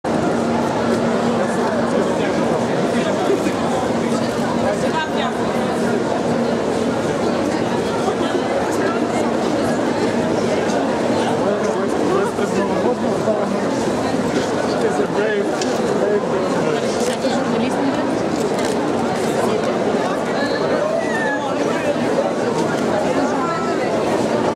Синютка підпрацьовує фотографом Порошенка
ВІдкриття заводу "Бадер" в с. Кожичі Яворівського району Львівської області